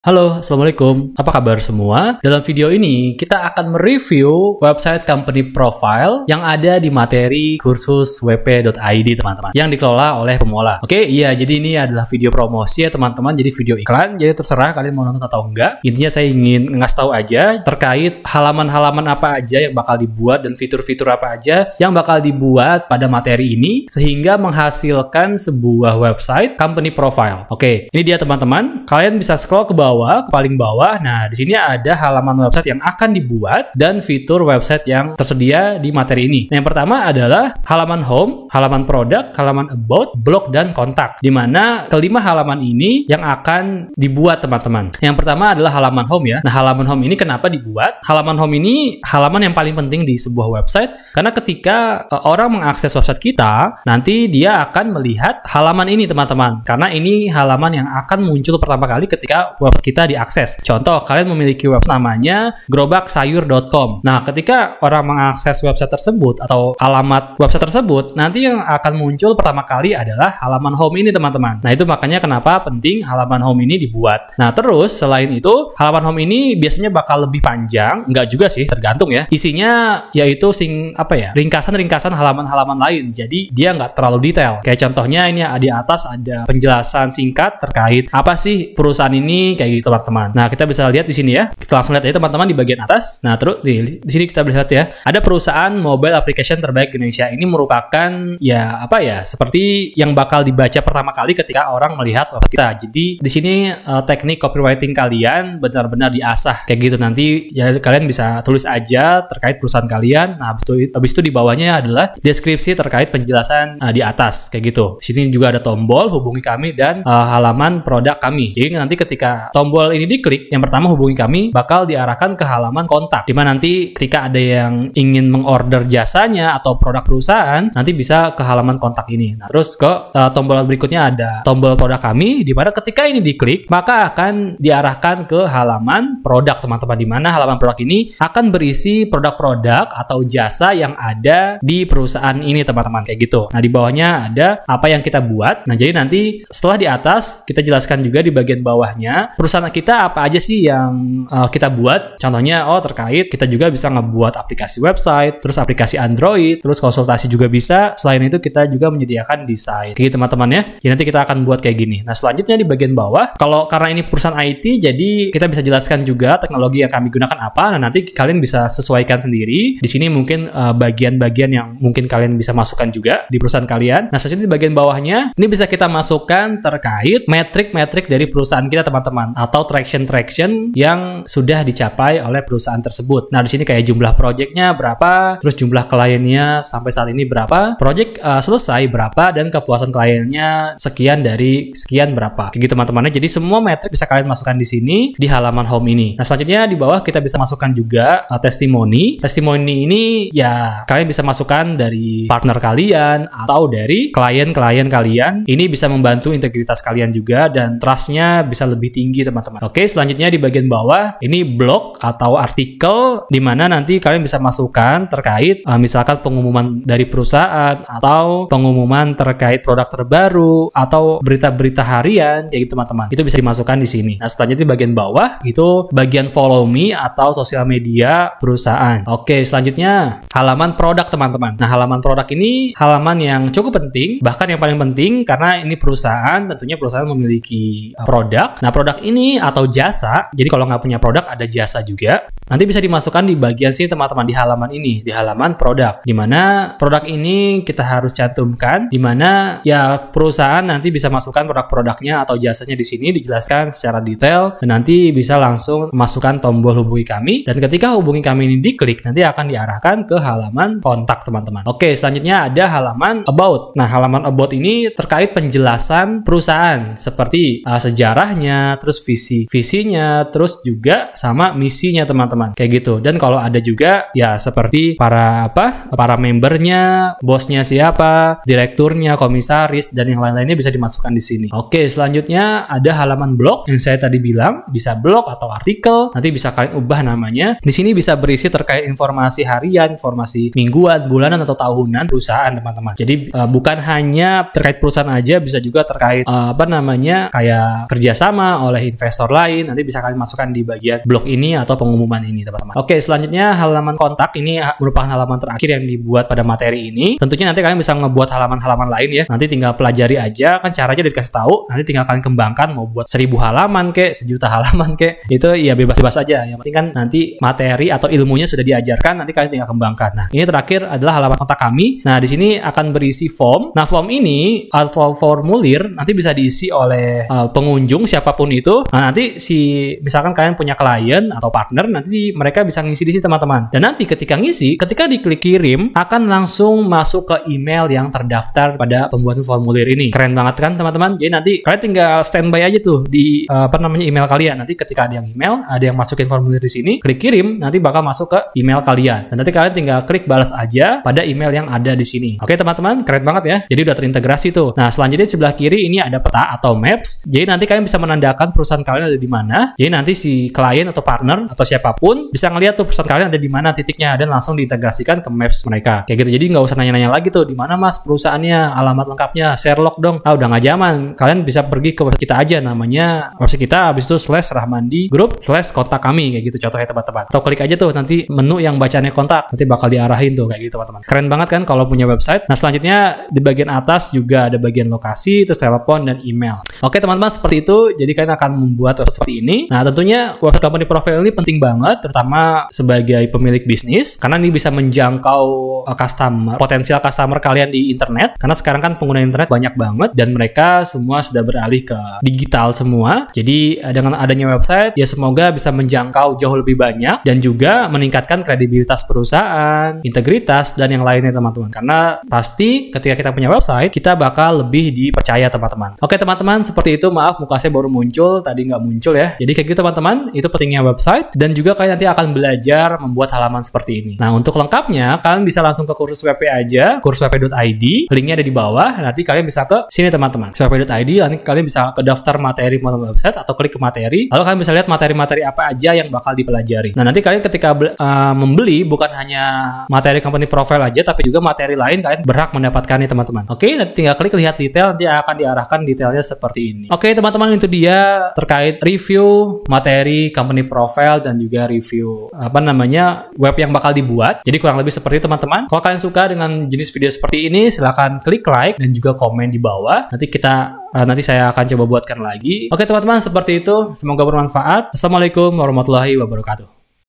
Ini Dia Website Company Profile Perusahaan, Client Auto Cenat-Cenut.
Review Website Company Profile untuk Peusahaan yang mau Go-Online dan ningkatin kredibilitas.

Group Diskusi, Instagram Pemoola, dan lainnya.